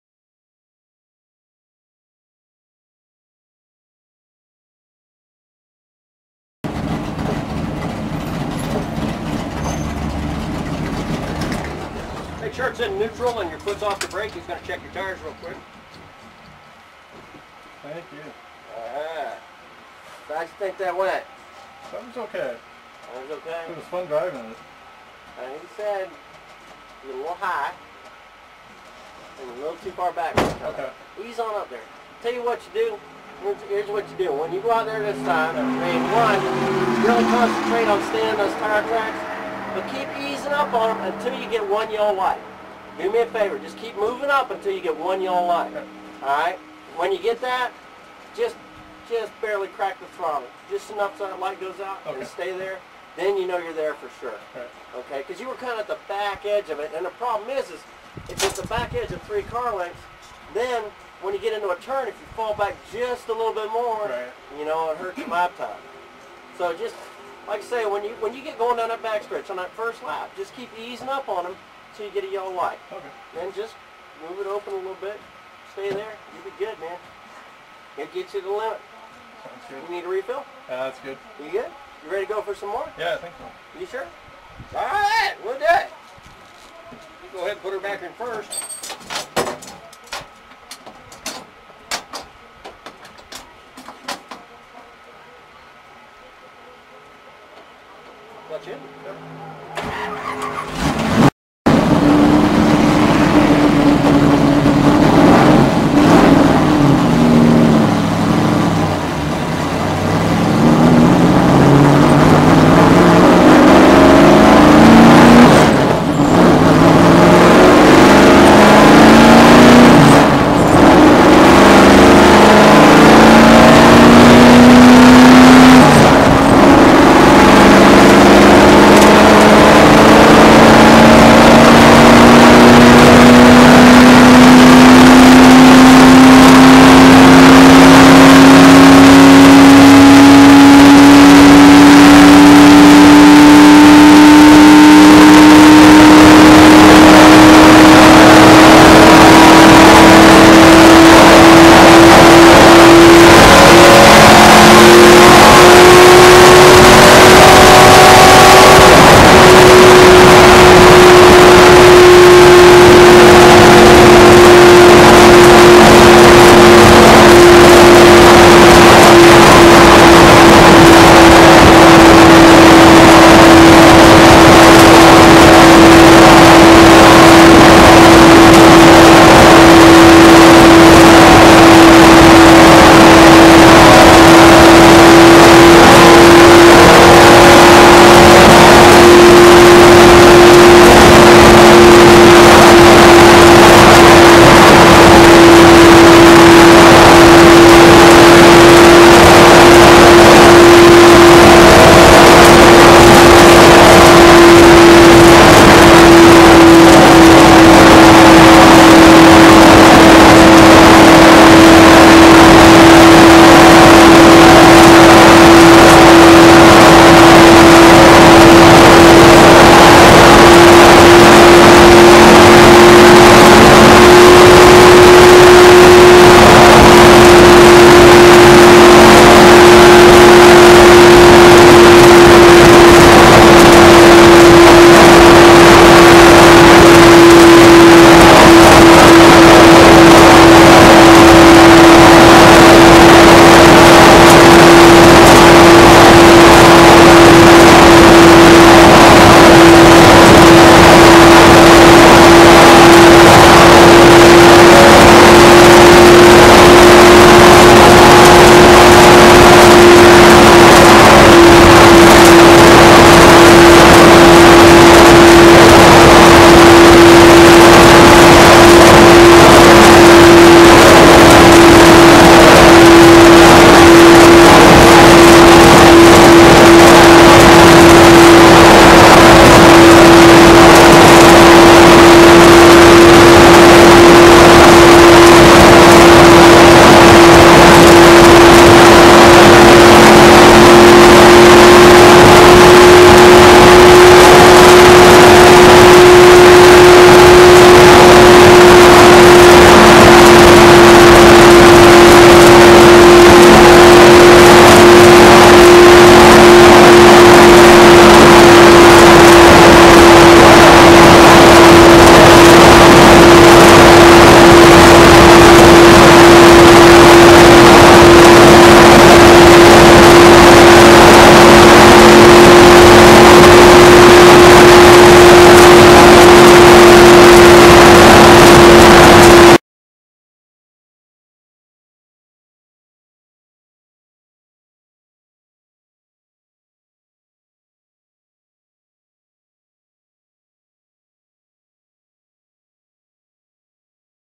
RPDE Driving at Daytona International Speedway
My turn driving laps 9 thru 16 @ Daytona. Engine start at about 1:25. What a thrill! Fastest lap was 66.3 and 146 mph top speed.